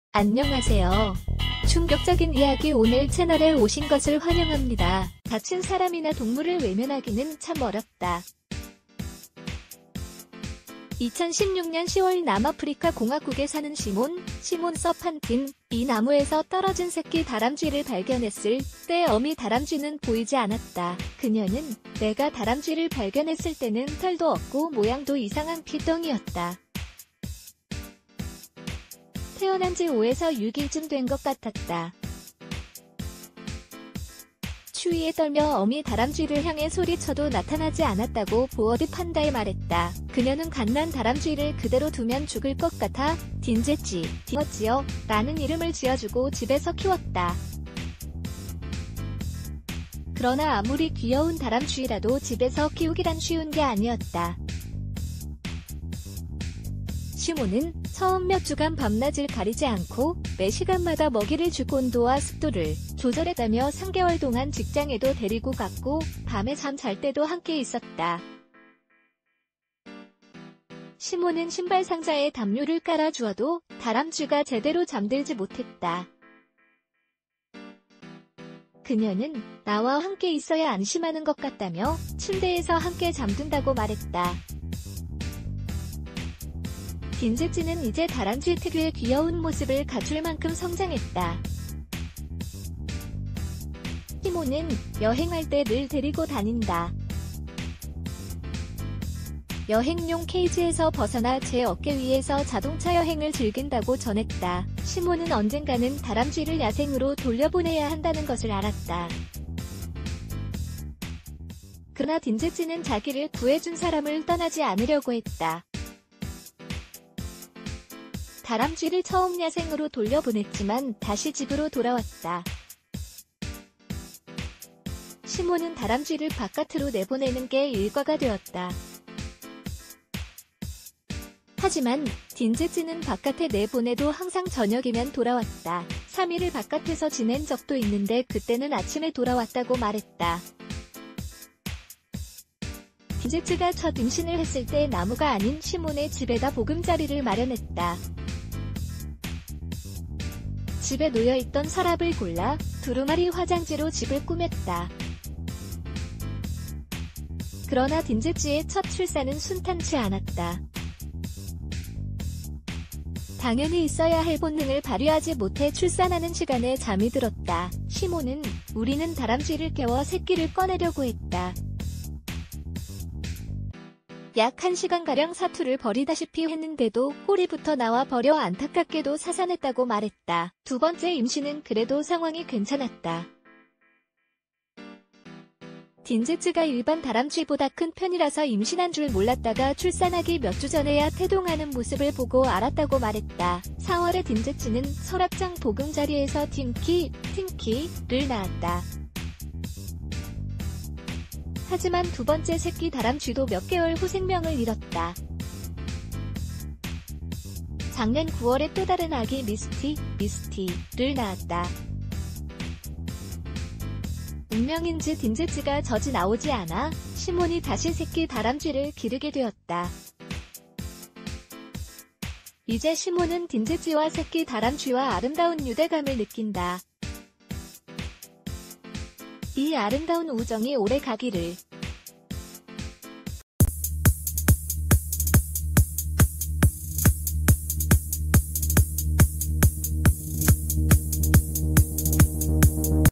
서랍에 보금자리 만든 귀여운 다람쥐
다친 사람이나 동물을 외면하기는 참 어렵다. 2016년 10월 남아프리카 공화국에 사는 시몬(Simone Serfontein)이 나무에서 떨어진 새끼 다람쥐를 발견했을 때 어미 다람쥐는 보이지 않았다. 그녀는 “내가 다람쥐를 발견했을 때는 털도 없고 모양도 이상한 핏덩이였다. 태어난 지 5~6일쯤 된 것 같았다. 추위에 떨며 어미 다람쥐를 향해 소리쳐도 나...